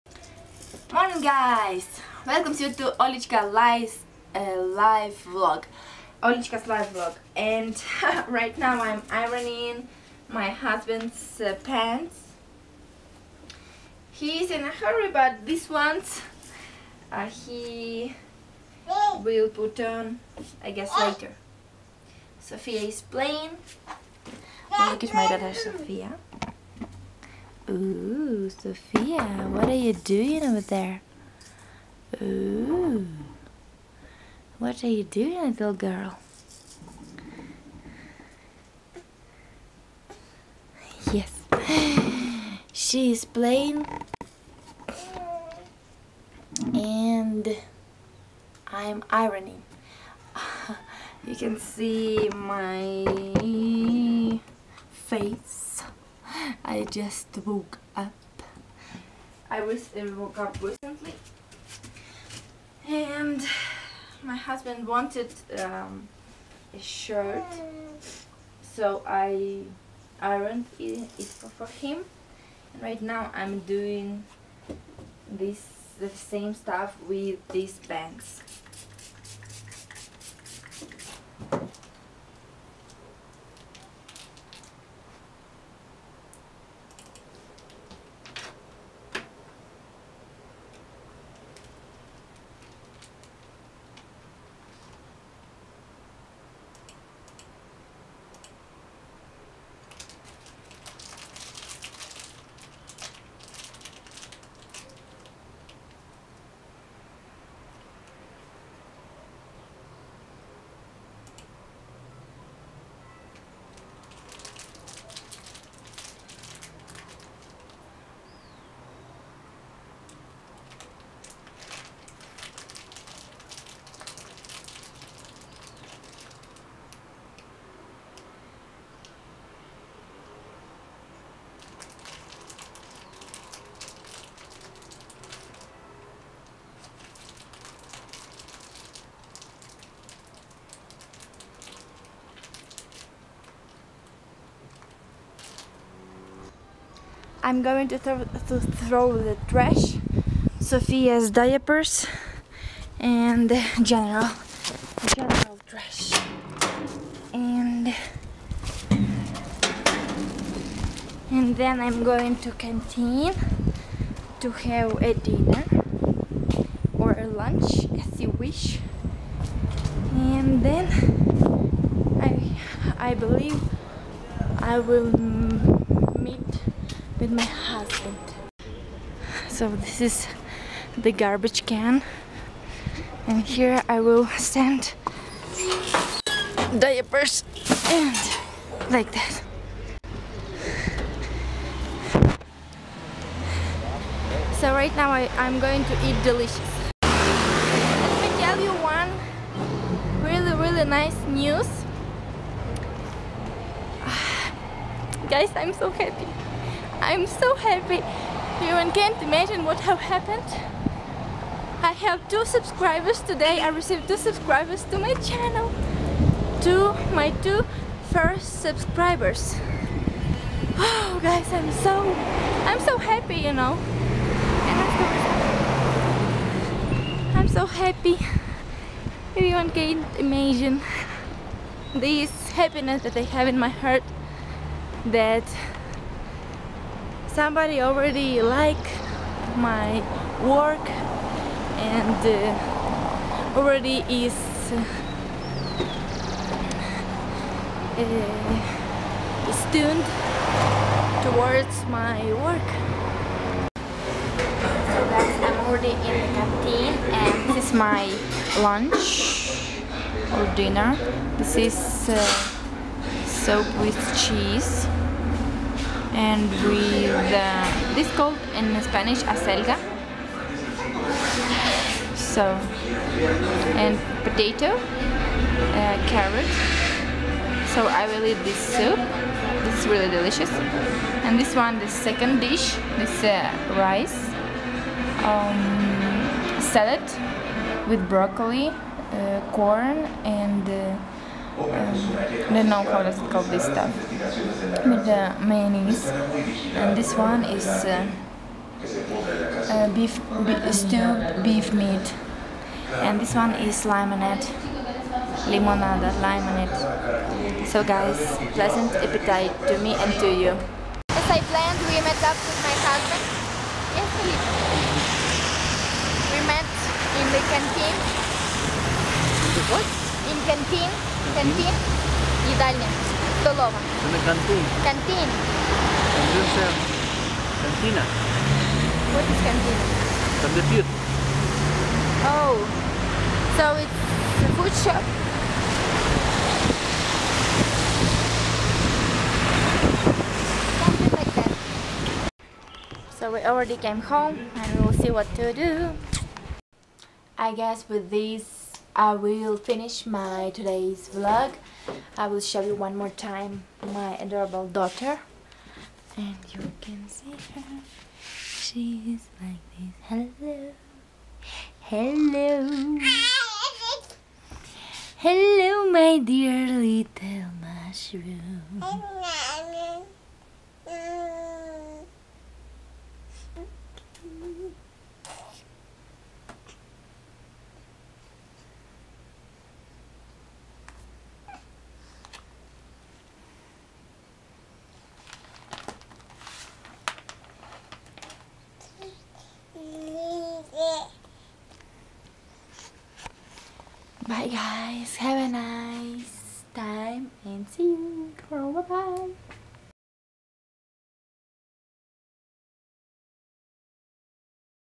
Happy news - My First Two Subscribers :D / Vlog Jan. 11, 2017
Hello everyone!

In this video you can see my daily activities and my Happiness towards my First Two Subscriptions ))

Hope you love and enjoy watching it! :D

Always yours - Olichka :)